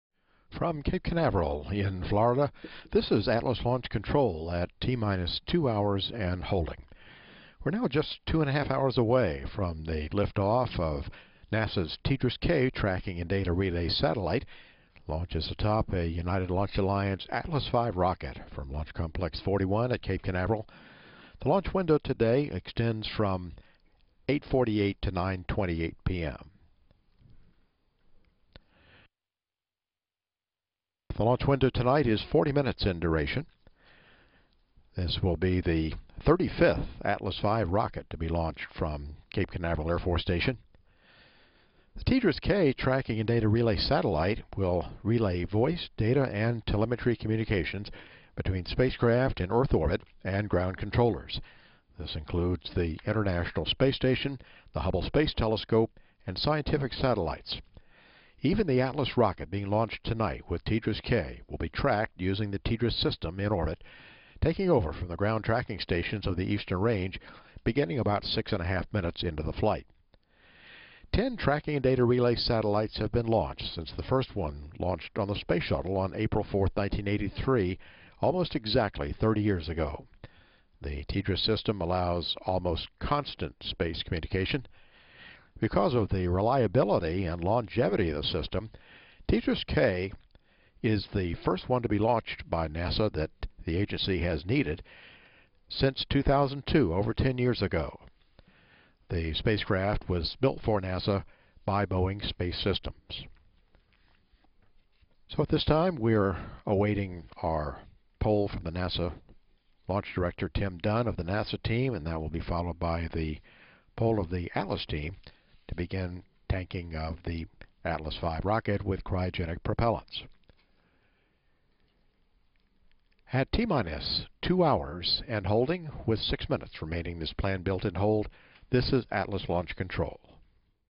Atlas V Rocket Launch Countdown
Atlas V/TDRS-K Launch Countdown.  NASA Launch Commentator George Diller opens the countdown coverage for the Atlas V launch of TDRS-K.